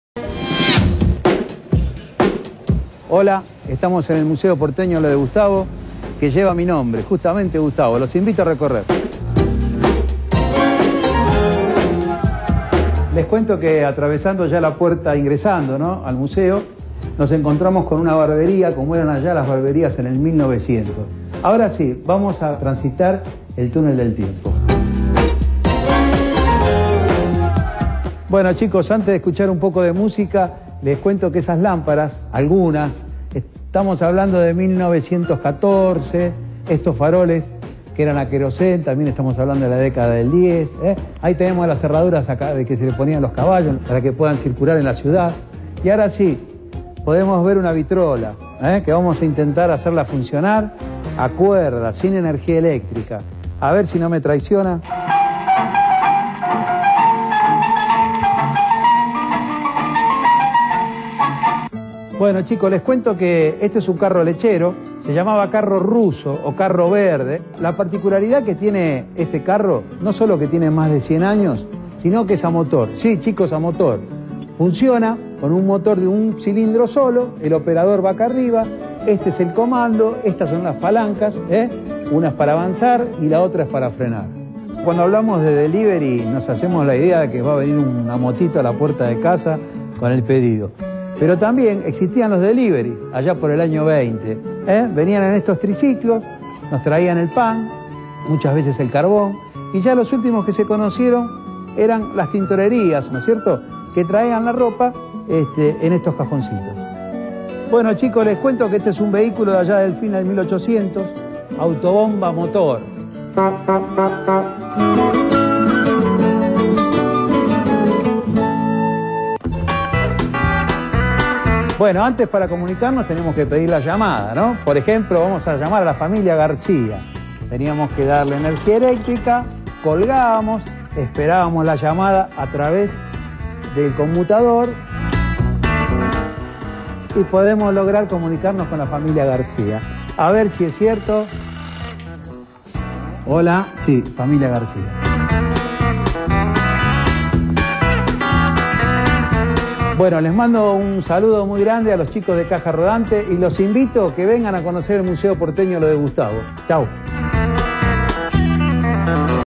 Caja rodante 11-01-11 Agenda. Curiosidades en el Museo porteño
Entramos al Museo porteño y conocemos las curiosidades de la historia argentina contada por los objetos tradicionales de cada época.